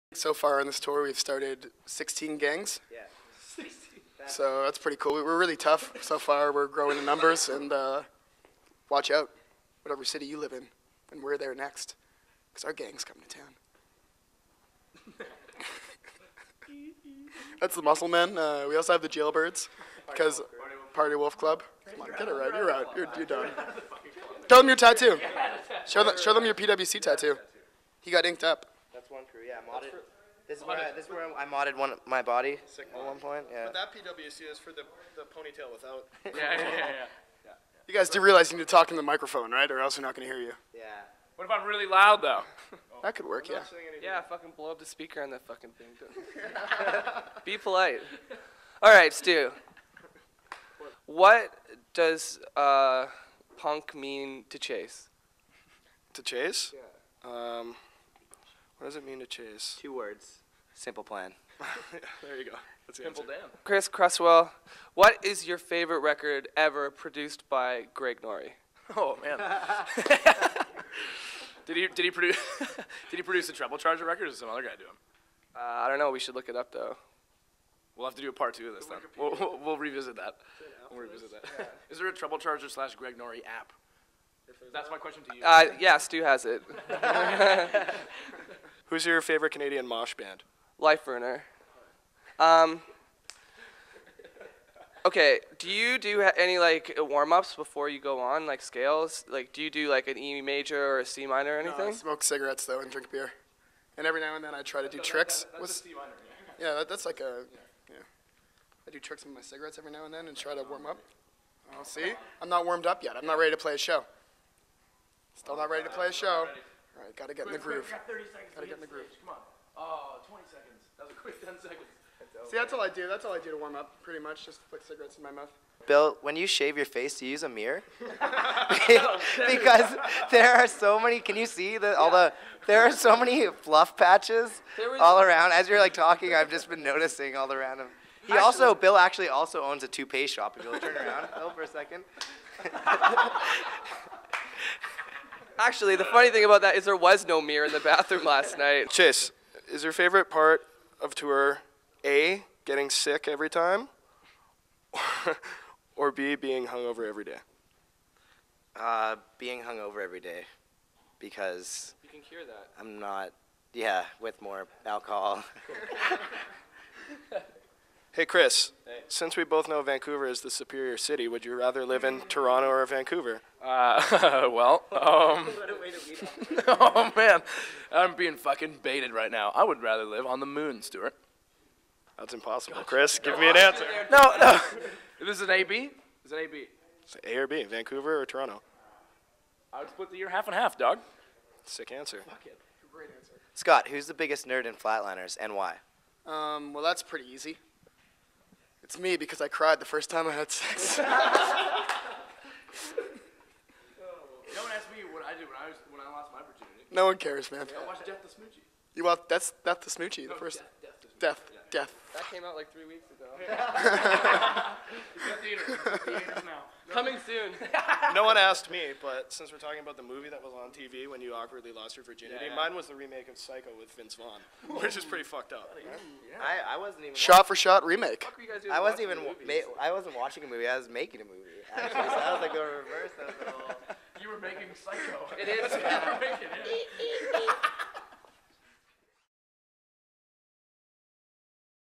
The Flatliners Interview Living With Lions
Vancouver's Living With Lions and Toronto's The Flatliners teamed up for a short tour of Central Canada, and what better way to get to know one another than interviewing each other?